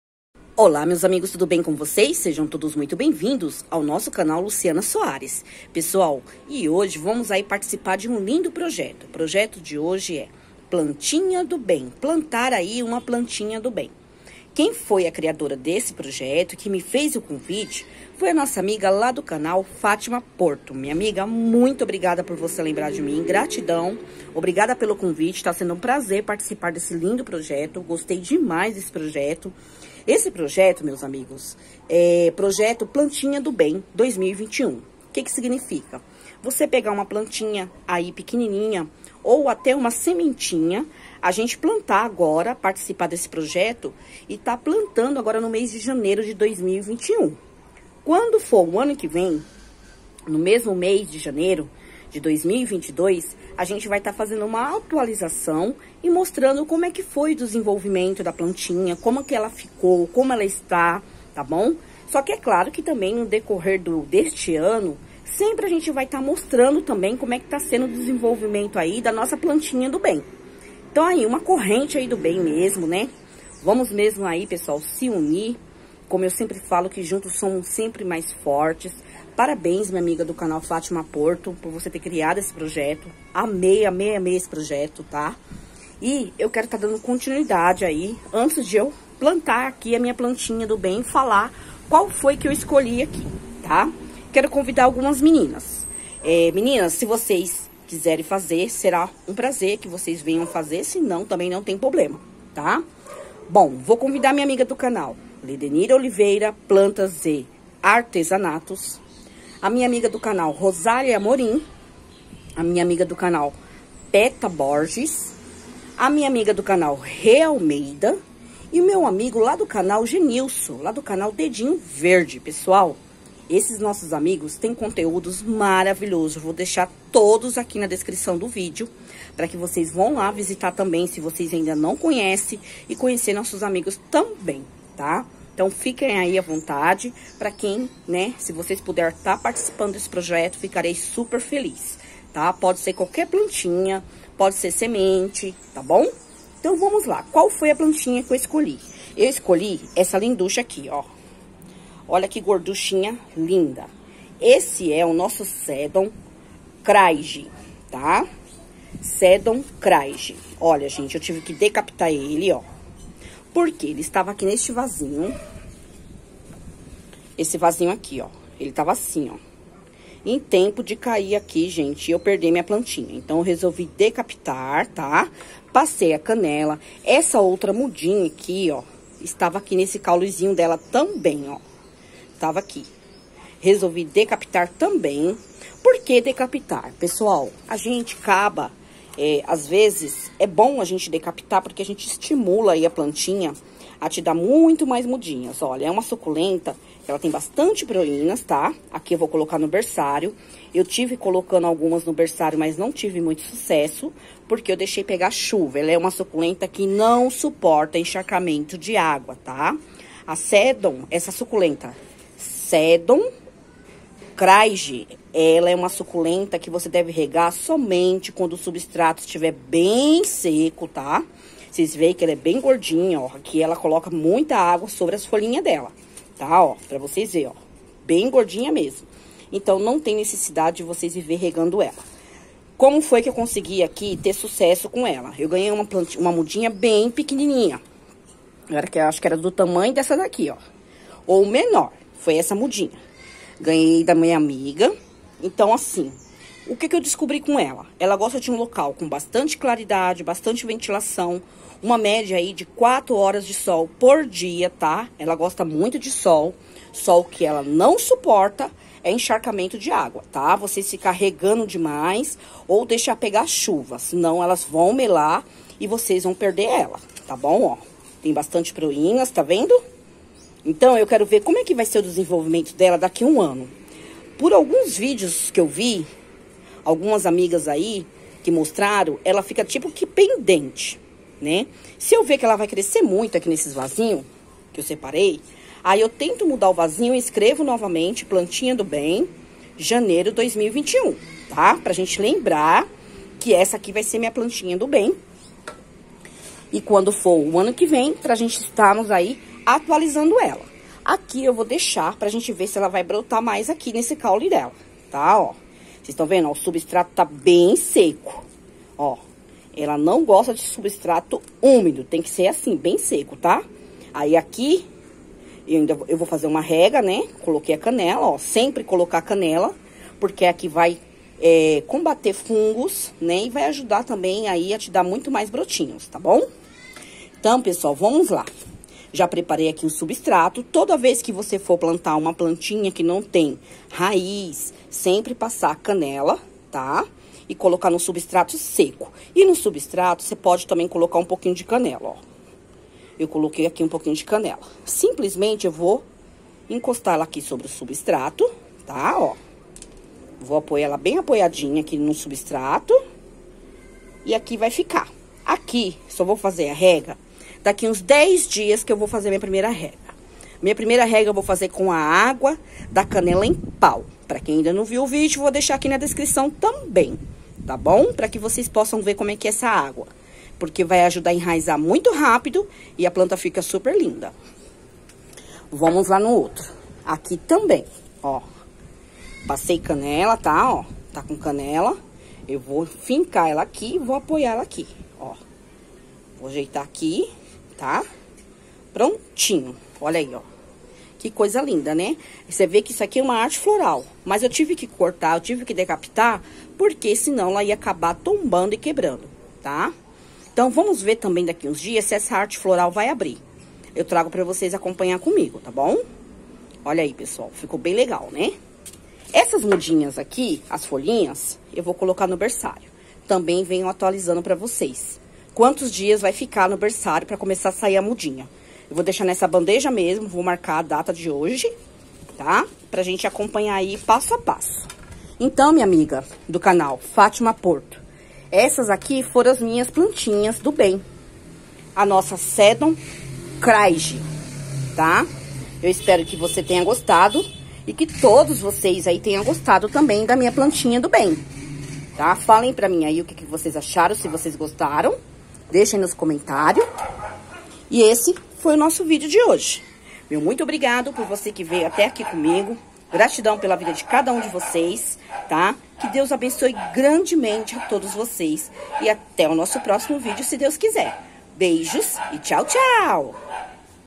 Projeto: Plantinha do Bem/2021
Olá meus amigos e amigas
No vídeo de hoje apresento a vocês a participação de um lindo projeto, plantar uma plantinha do bem
Um projeto que foi criado pela nossa amiga do canal Fátima Porto, que fui convidada por ela mesma...

A minha plantinha do bem e a nossa Suculenta sedum craigii , uma Suculenta de fácil Cultivo, más que não suporta encharcamento de água no seu substrato, ela gosta de um local com muita claridade e de no mínimo 4 horas de sol por dia, suas regas devem ser feita somente quando o substrato estiver bem seco...

Espero pode ajudar a cada um de vocês com essas dicas de cultivo de hoje...

Muito obrigada pelo carinho e atenção de todos vocês, fiquem todos com Deus, gratidão...